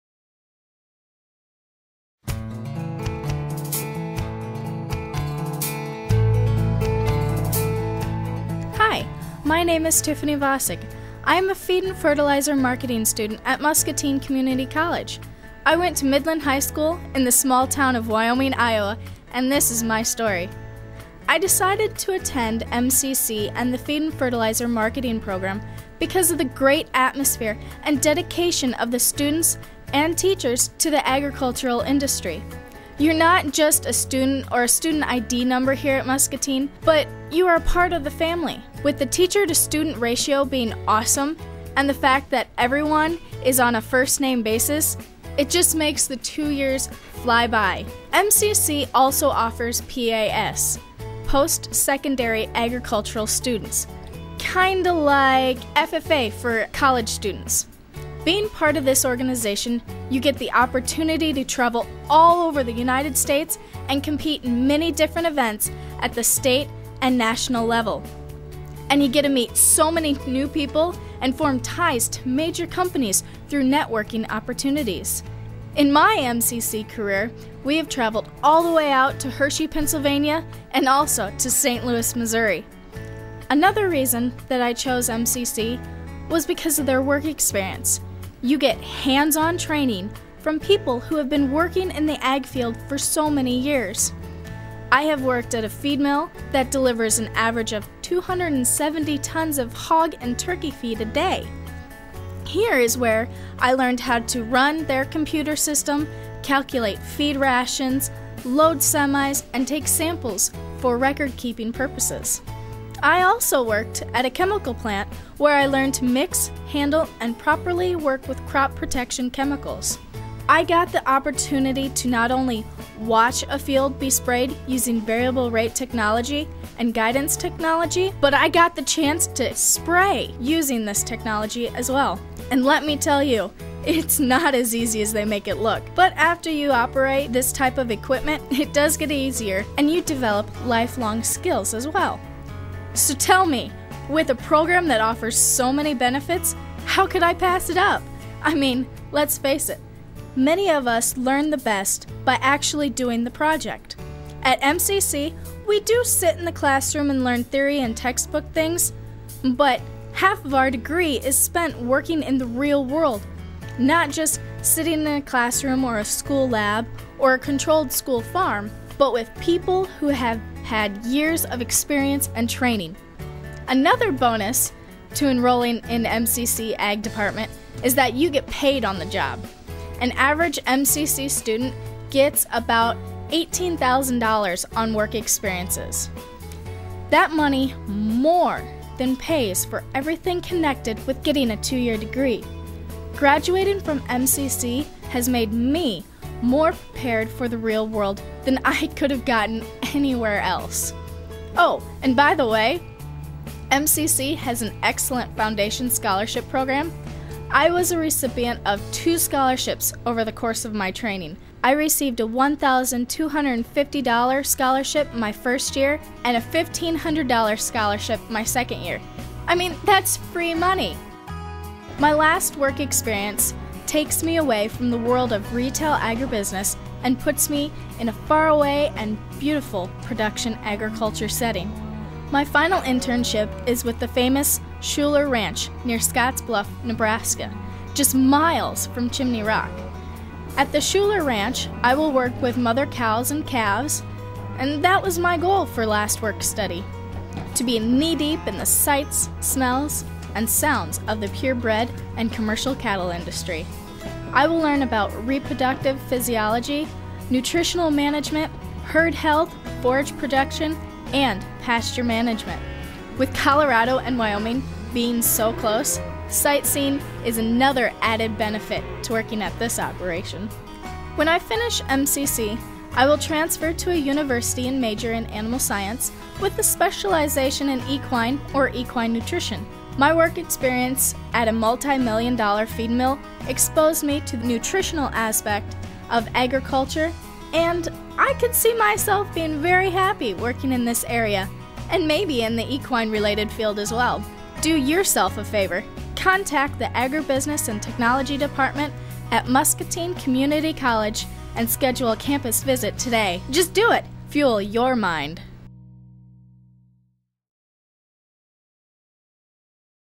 MCC Agriculture Programs
Muscatine Community College, Iowa, offers ag programs in Farm Management, Feed and Fertilizer Marketing, Horticulture and Veterinary Technology. When they say their programs are hands-on, they mean it. A large part of the students' training is in the field where they not only earn valuable experience, but real money. Students often earn enough on the job to pay for their education.